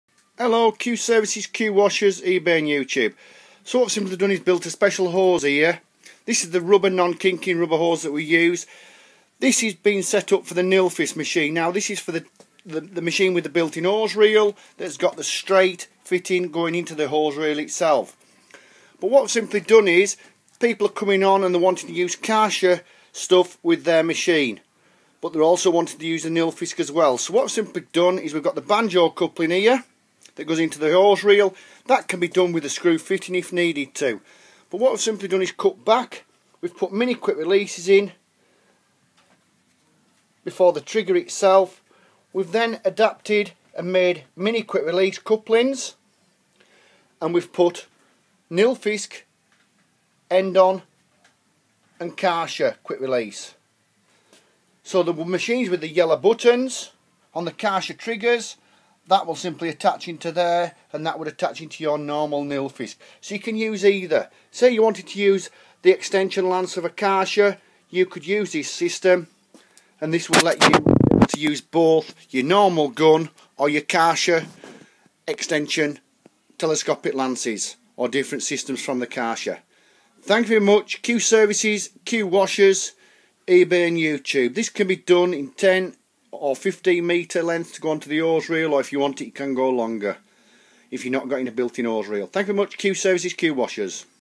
PRESSURE WASHER OLD TYPE NILFISK BUILT IN STAINLESS BANJO TO KARCHER OR NILFISK PROBES QUICK RELEASE
THIS IS A HOSE THAT FITS THE OLD TYPE NILFISK BUILT IN HOSE REEL TO LET YOU USE NILFISK TRIGGER OR QUICK RELEASE TO KARCHER TRIGGER OR TELESCOPIC LANCE EXTENDABLE OR UNDER BODY WASH QWASHERS @ EBAY, FACEBOOK OR YOUTUBE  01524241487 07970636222 Q SERVICES LTD SALES SERVICES PARTS & ACCESSORIES INGLETON LANCASHIRE NORTH YORKSHIRE